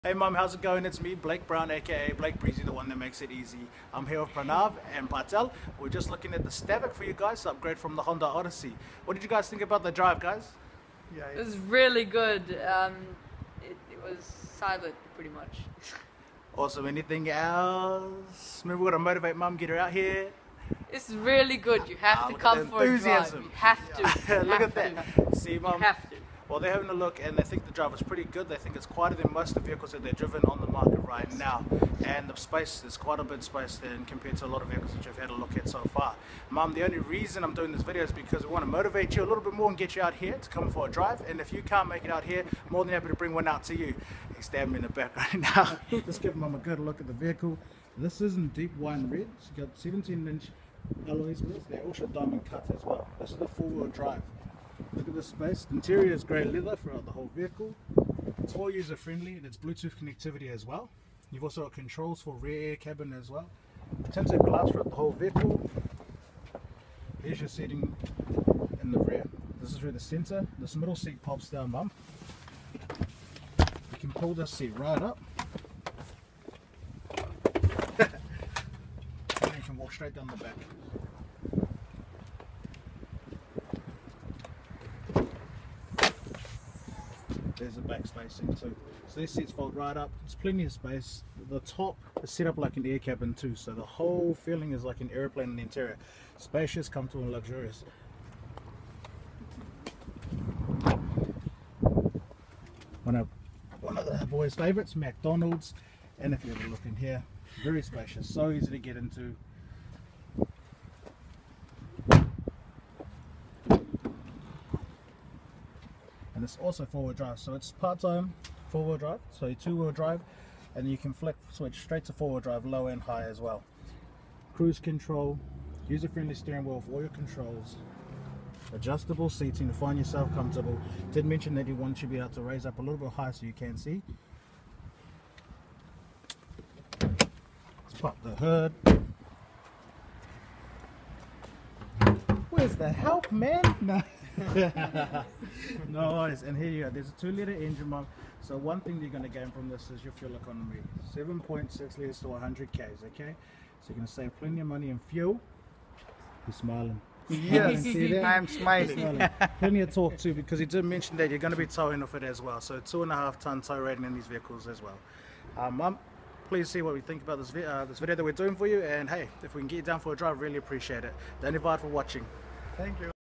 Special walk around with the Patel family for mum at home - Brand New 2015 SsangYong Stavic SPR
Hey mum, we understand how busy you must be as the school holidays have just finished and where all back in the rat race but we thought we would put together a little video so you can see the vehicle and what your family believes could be the right vehicle for you and the family so I challenge you to come down and go for a drive with me Blake Breezy the one that makes it's easy!